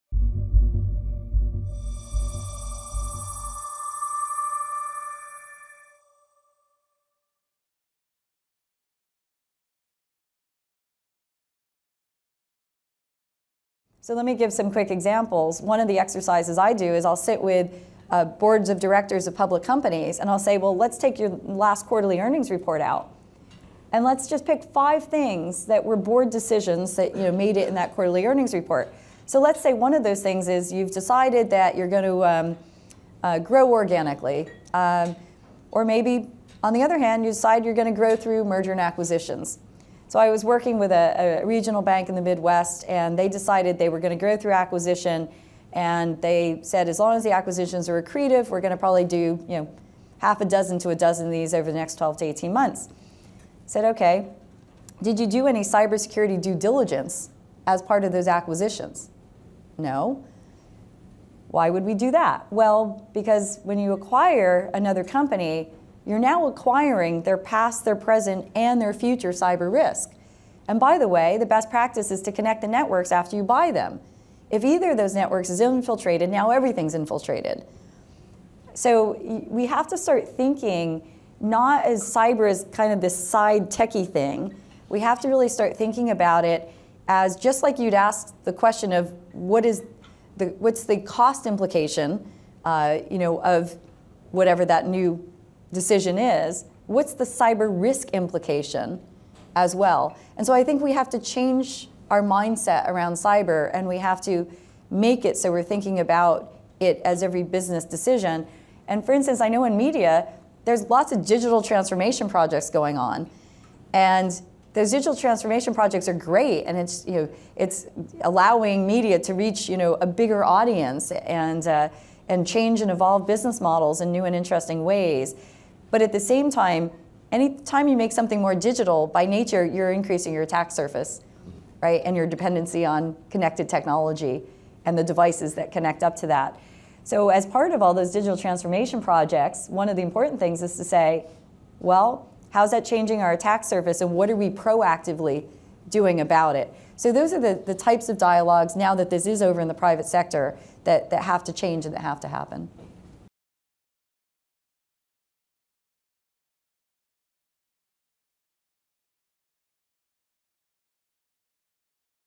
AI and Cybersecurity: A Conversation with Nicole Eagan
Nicole Eagan, Chief Executive Officer, Darktrace
Moderator: Kevin Collier, Cybersecurity Correspondent, BuzzFeed News